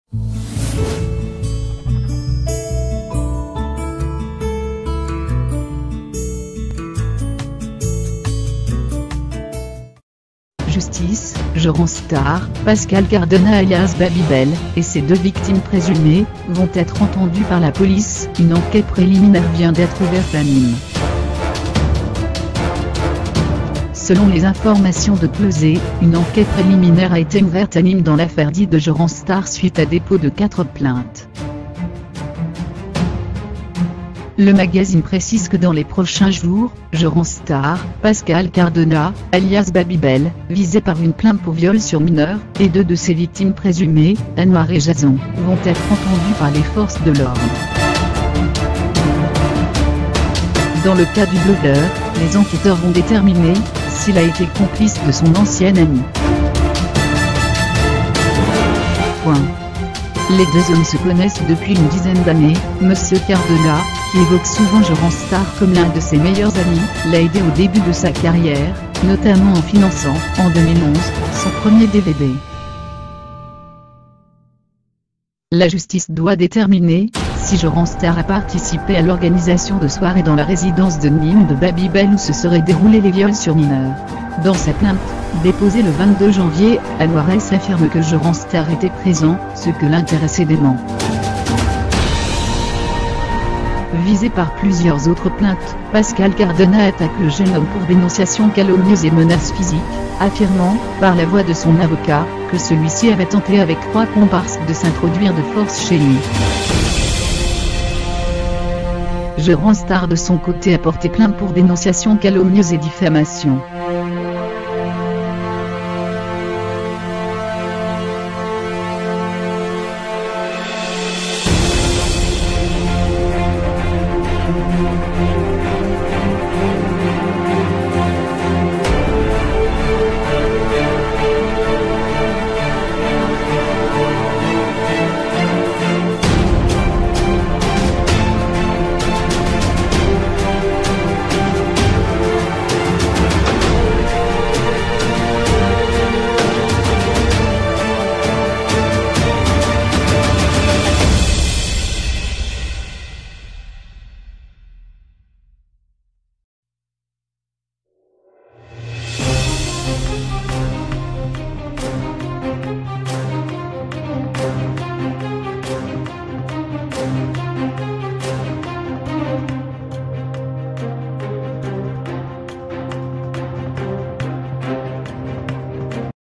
Justice: Jeremstar, Pascal Cardonna alias Babybel et ses 2 victimes présumées vont être enten
Justice: Jeremstar, Pascal Cardonna alias Babybel et ses 2 victimes présumées vont être entendus par la police - Une enquête préliminaire vient d'être ouverte à Nîmes
Selon les informations de Closer, une enquête préliminaire a été ouverte à Nîmes dans l'affaire dite de Jeremstar suite au dépôt de quatre plaintes.